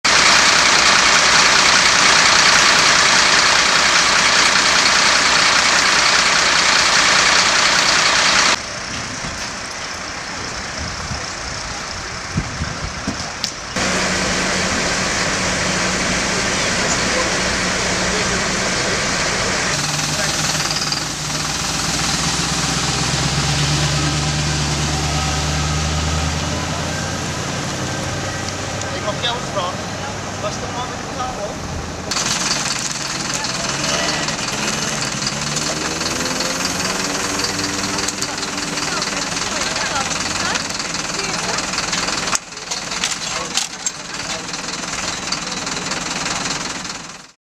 Auto over de kop na botsing tegen boom op Vondelweg in Haarlem
Haarlem - Een automobilist is zondagmiddag gewond geraakt bij een verkeersongeval. Het ongeval vond om even over half vier plaats. Door nog onbekende oorzaak kwam de automobilist aan de andere kant van de weg terecht, reed de stoep op en knalde tegen een boom aan. Vervolgens vloog de auto over de kop en kwam enkele meters verder tot stilstand.